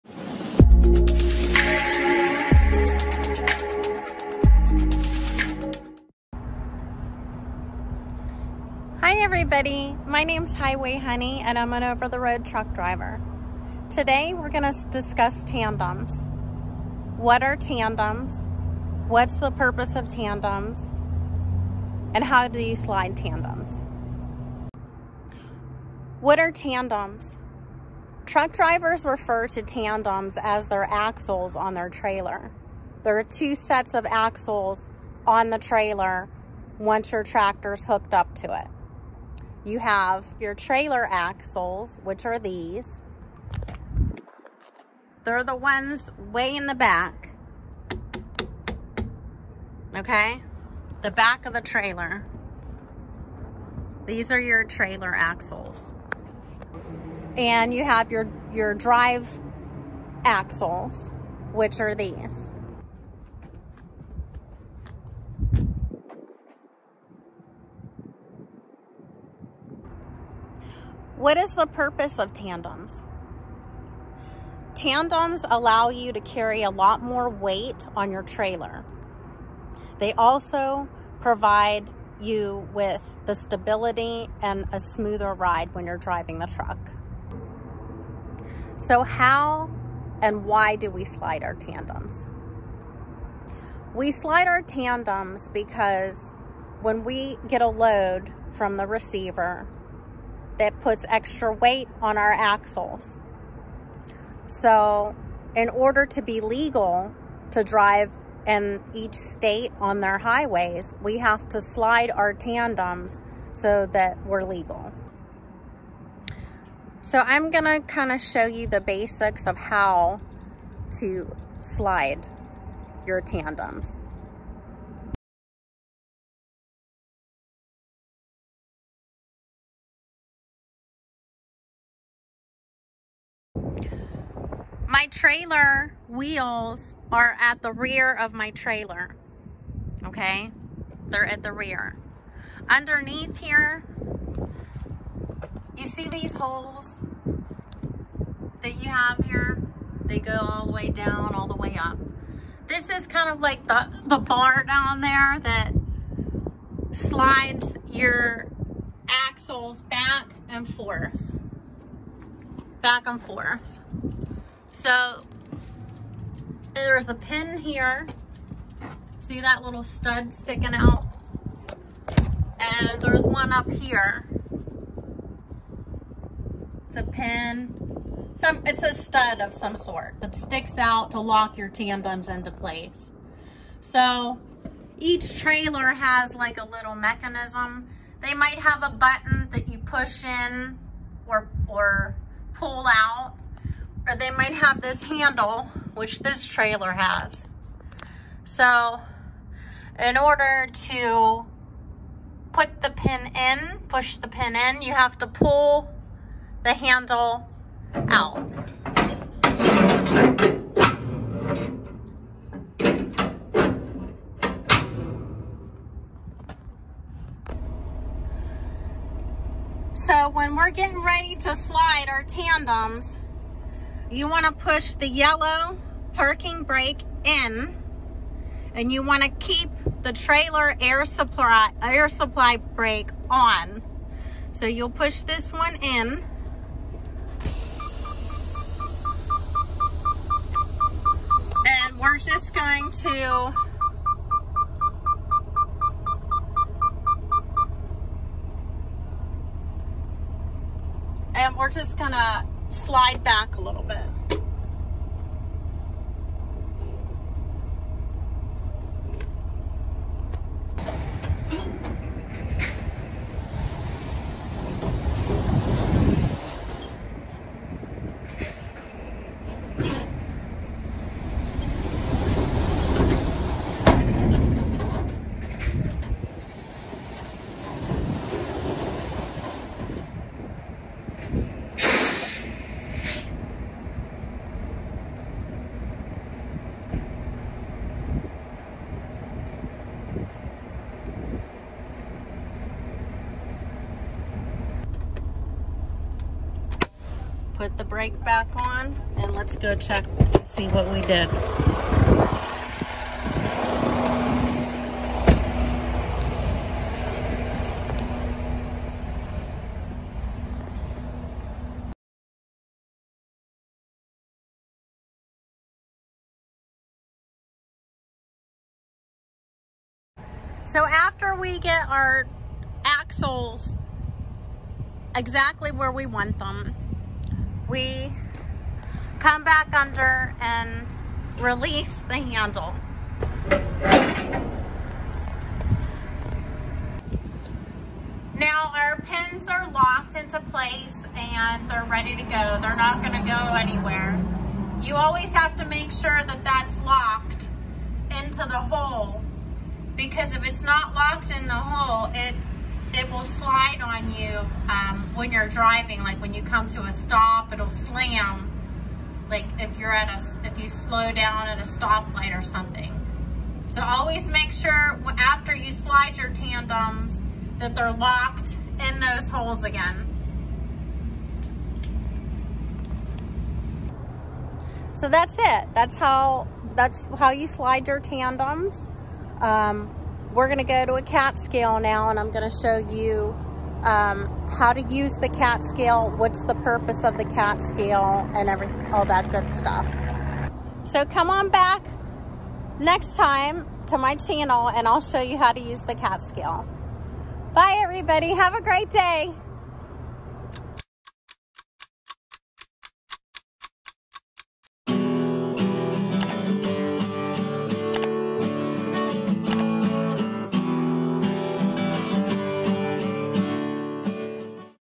HOW TO SLIDE TANDEMS…
Sliding tandems is necessary and required for three main things: To adjust move some weight off of the tractor's axles rear (drive axles) to the trailer tandem axles. To adjust weight OFF of the trailer axles and move some of the weight forward to the tractor axles. (Steer axles and the drive axles)

By sliding the trailer tandems forward, you will put more weight on the trailer tandems and take weight off the tractor's drive axles. By sliding the trailer tandems toward the rear of the trailer, you will take weight off the trailer tandems and put more weight onto the tractor's drive axles.

On my next video on the Cat scale I will go a little deeper on weights  for each axel.

Honey ❤️